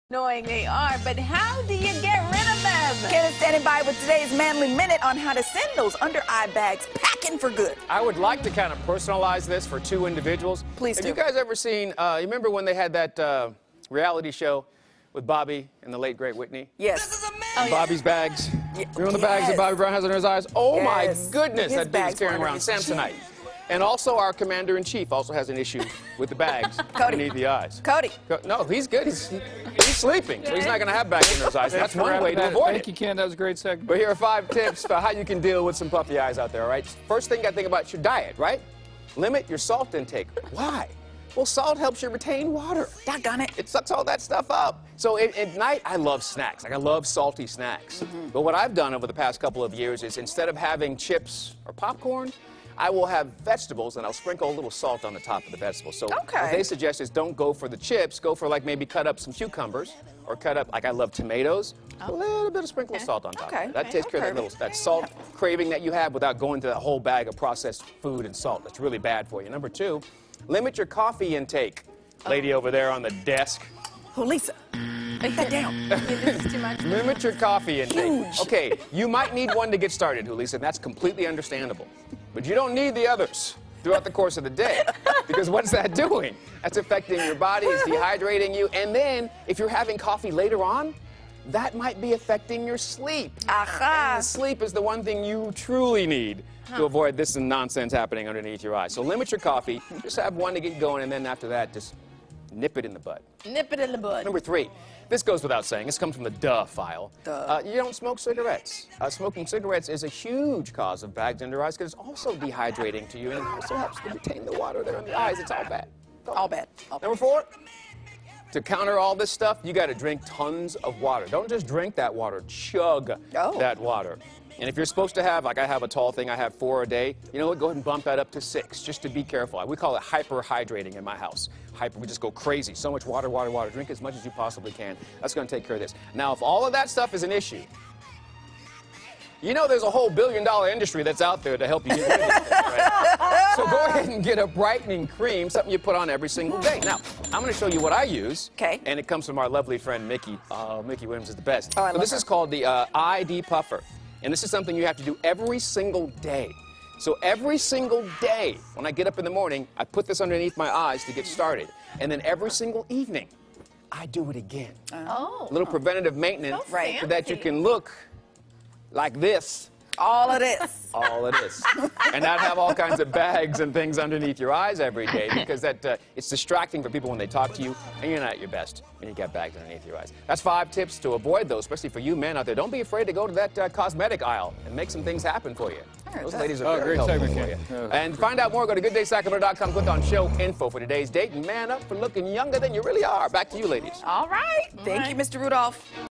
Manly Minute: Puffy Eyes
In today's Manly Minute, Ken has five tips for puffy eyes.